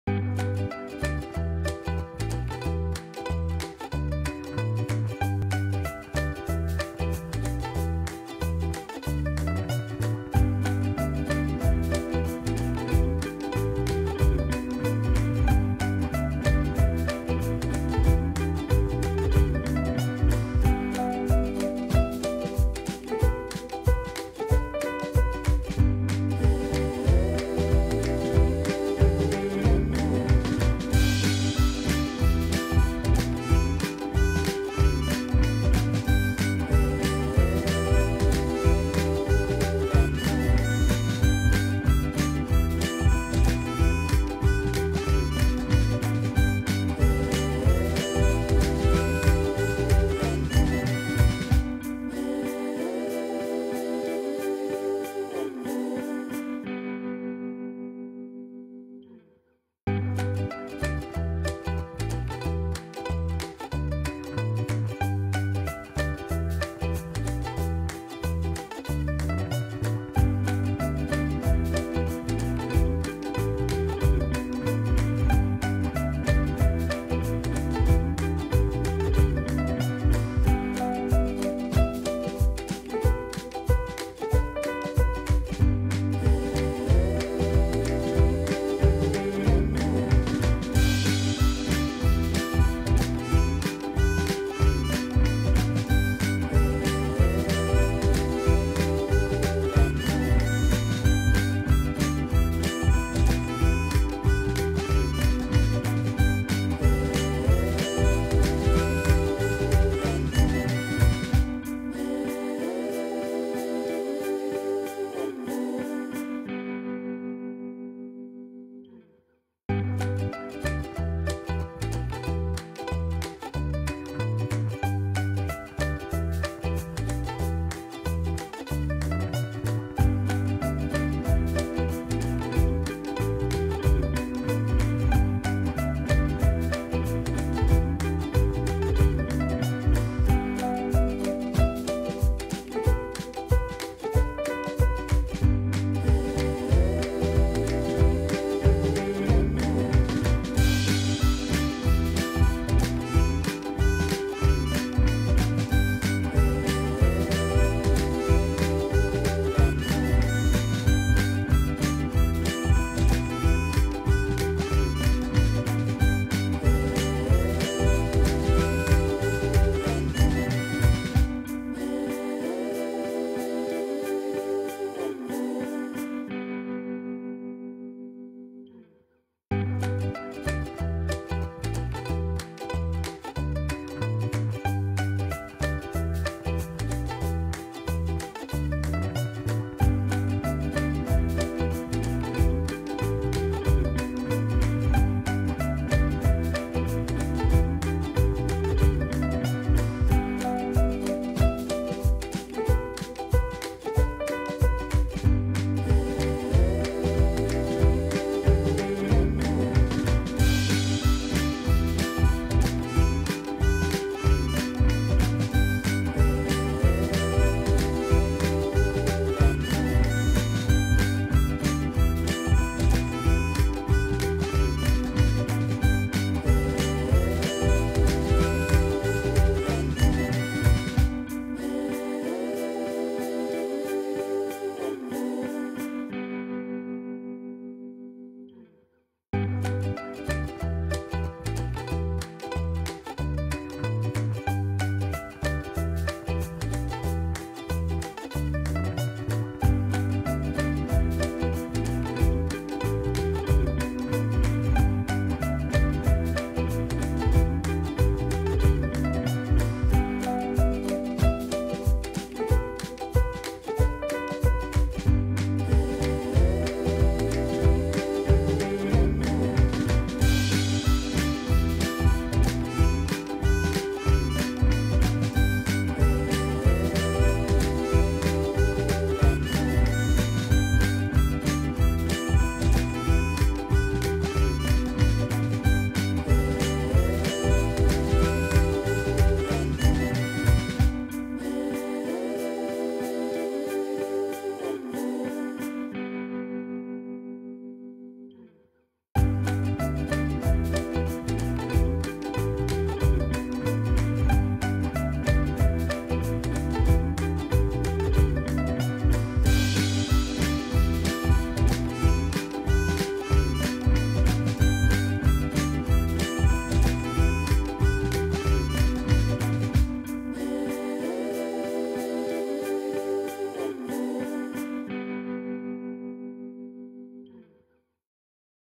Обзор сборной модели 32062 AH-1G Cobra с американскими вертолетчиками (война во Вьетнаме)
арт.32062 1:32